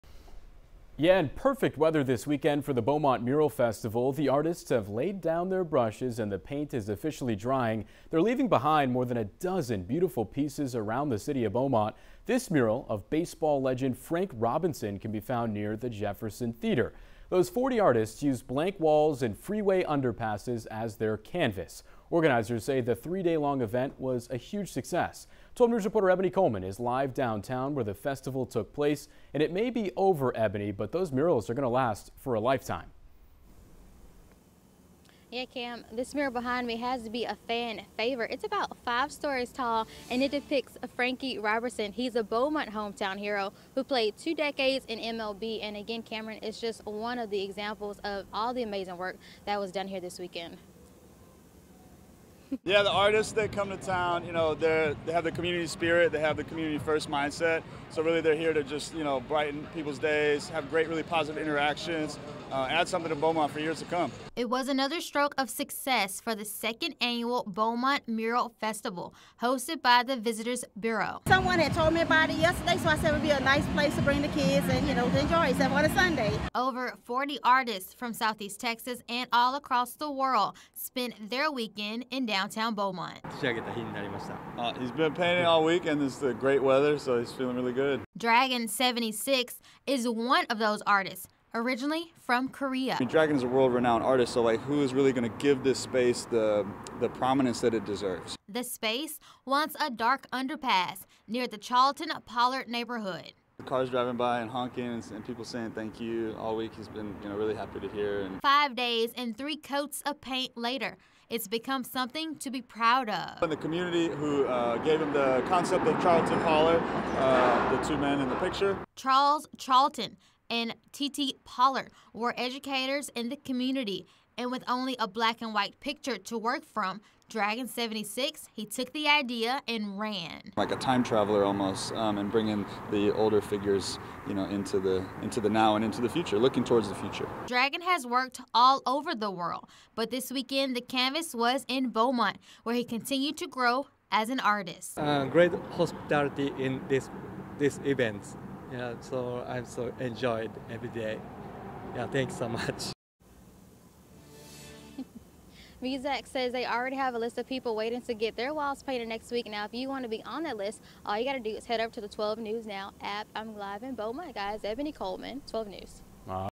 Art created during second annual Mural Festival honors Beaumont's past while looking forward to the
During Beaumont Mural Festival, the artists used blank walls and freeway underpasses as their canvases.